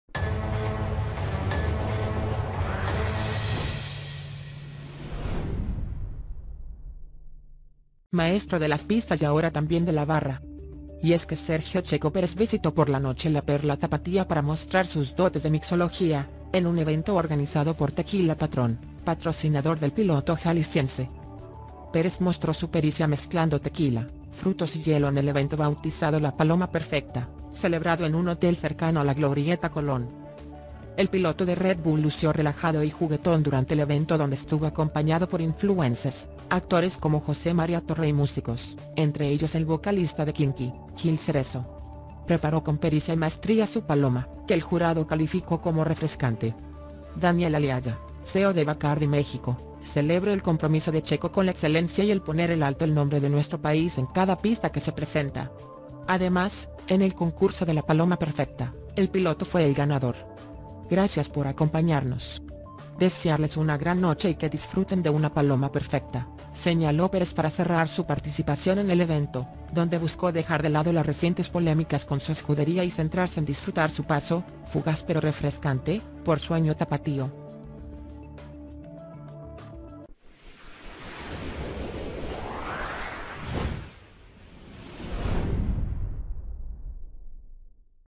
Checo Pérez asombra por su talento en la mixología durante evento en la Perla Tapatía
Maestro de las pistas y ahora también de la barra. Y es que Sergio "Checo" Pérez visitó por la noche la Perla Tapatía para mostrar sus dotes de mixología, en un evento organizado por Tequila Patrón, patrocinador del piloto jalisciense.

Pérez mostró su pericia mezclando tequila, frutos y hielo en el evento bautizado La Paloma Perfecta, celebrado en un hotel cercano a la glorieta Colón.

El piloto de Red Bull lució relajado y juguetón durante el evento donde estuvo acompañado por influencers, actores como José Maria Torre y músicos, entre ellos el vocalista de Kinky, Gil Cerezo. Preparó con pericia y maestría su paloma, que el jurado calificó como refrescante.

Daniel Aliaga, CEO de Bacardi México, celebro el compromiso de Checo con la excelencia y el poner el alto el nombre de nuestro país en cada pista que se presenta. Además, en el concurso de la Paloma Perfecta, el piloto fue el ganador.

"Gracias por acompañarnos. Desearles una gran noche y que disfruten de una Paloma Perfecta", señaló Pérez para cerrar su participación en el evento, dónde buscó dejar de lado las recientes polémicas con su escudería y centrarse en disfrutar su paso, fugaz pero refrescante, por sueño tapatío.